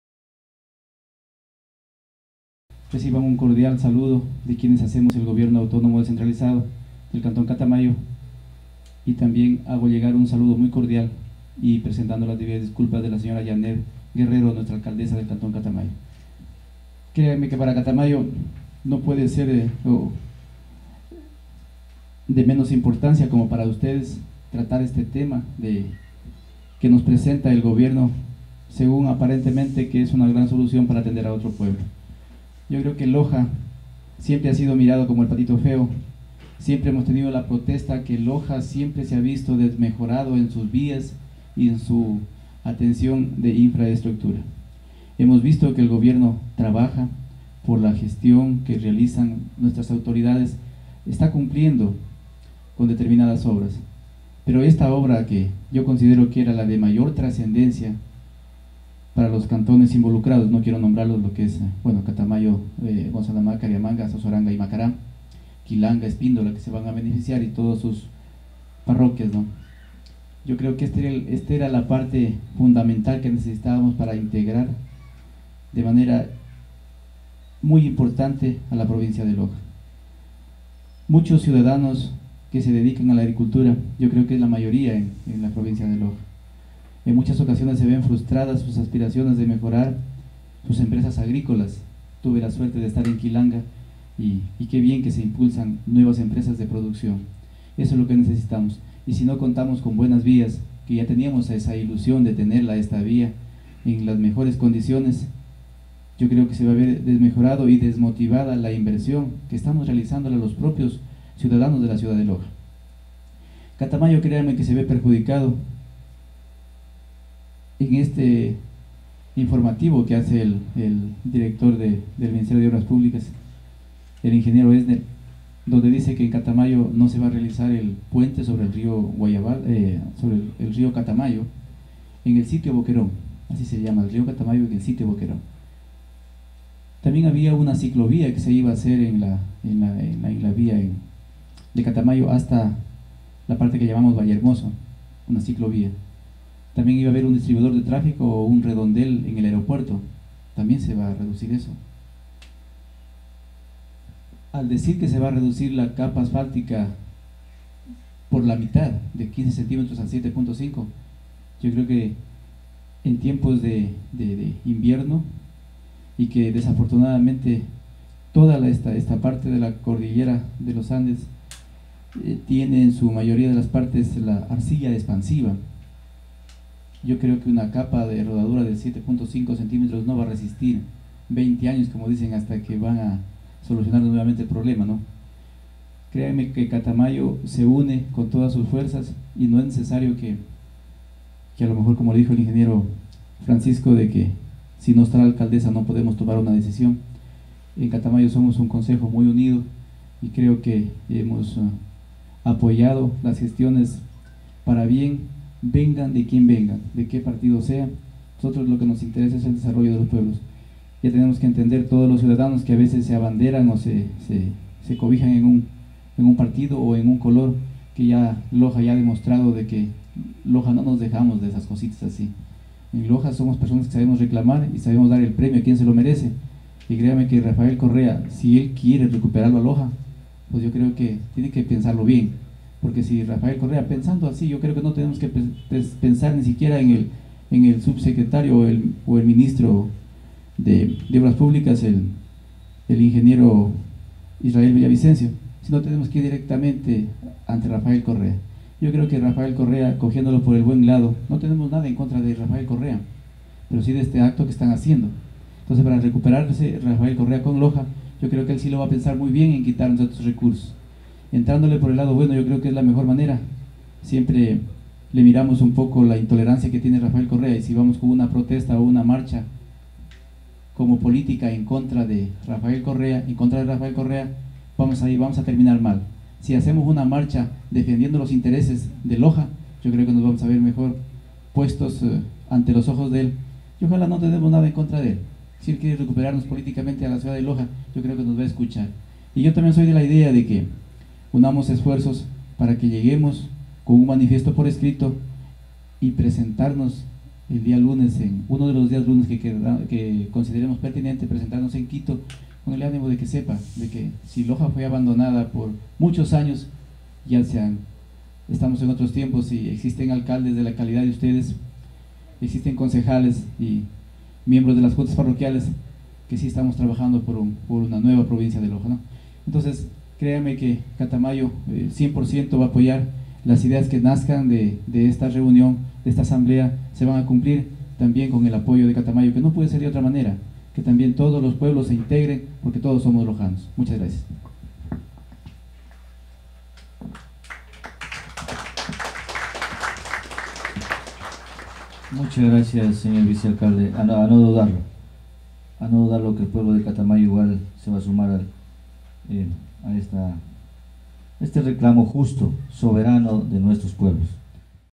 Catamayo.- Oswaldo Celi. Reunión de alcaldes en Sozoranga
- Reducción de presupuesto vías Catamayo - Macará
Cámara: Leonardo Piedra

Saludos,

Pepe Simancas
Community Manager